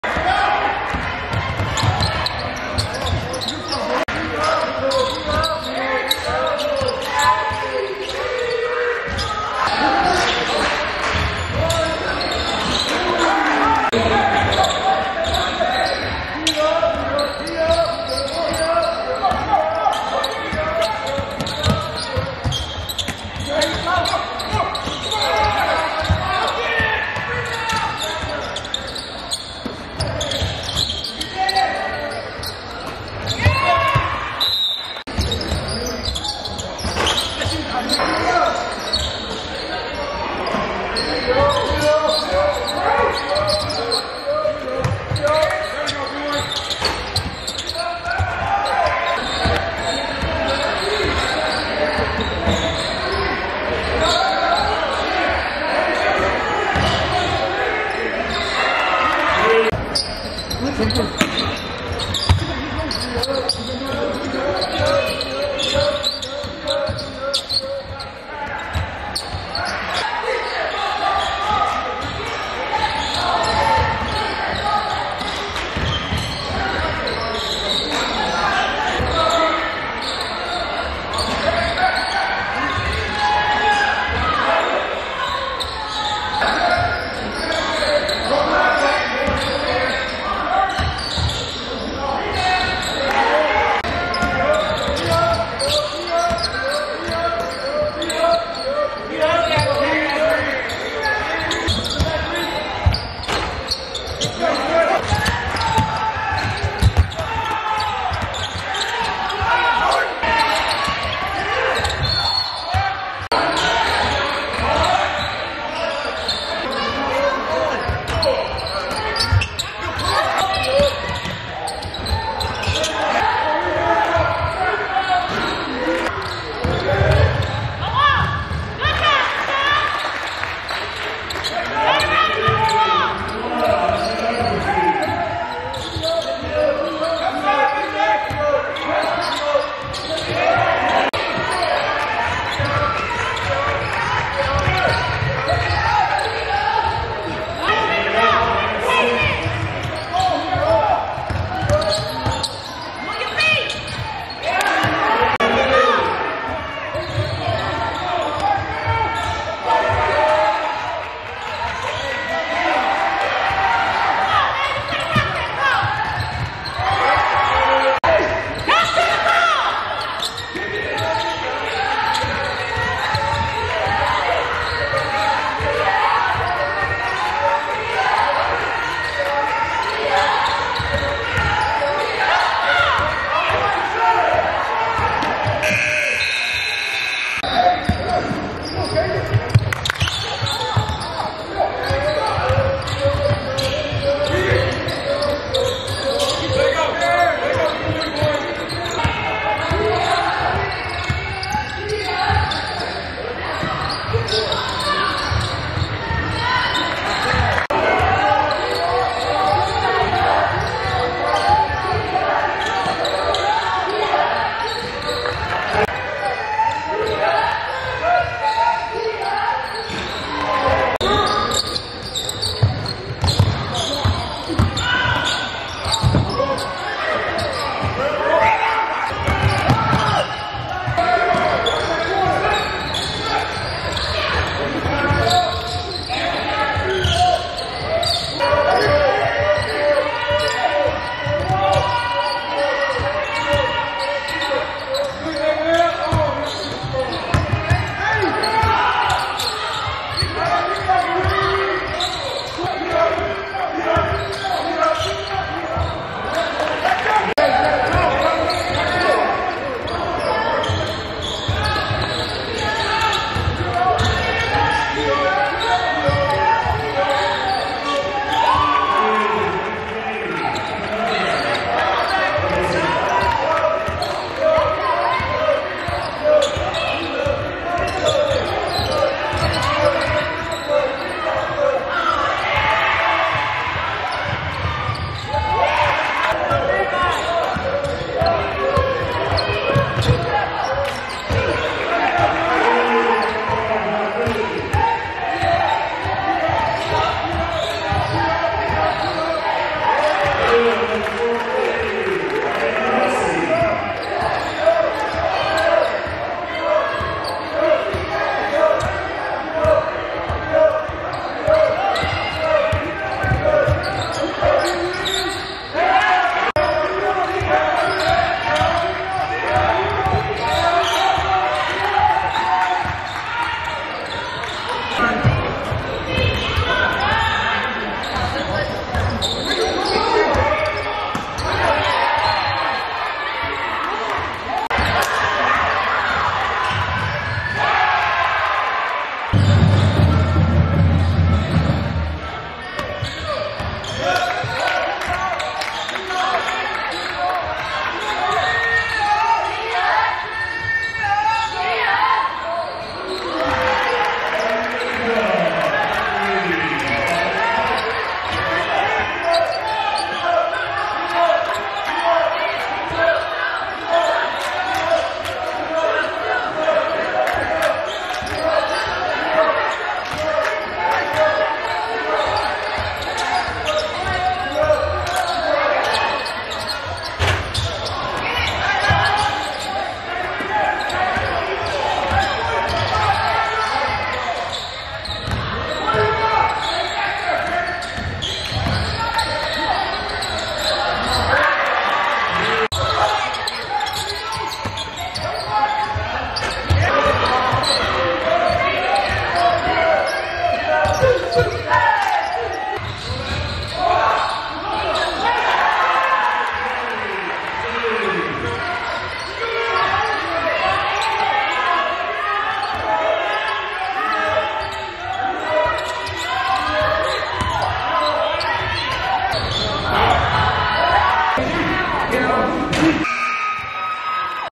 Can Rogers Final Win A Game Against East Central | Basketball 2022 - 2023
This is a basketball contest consider two TPS schools Will Rogers High School VS East Central Cardinals.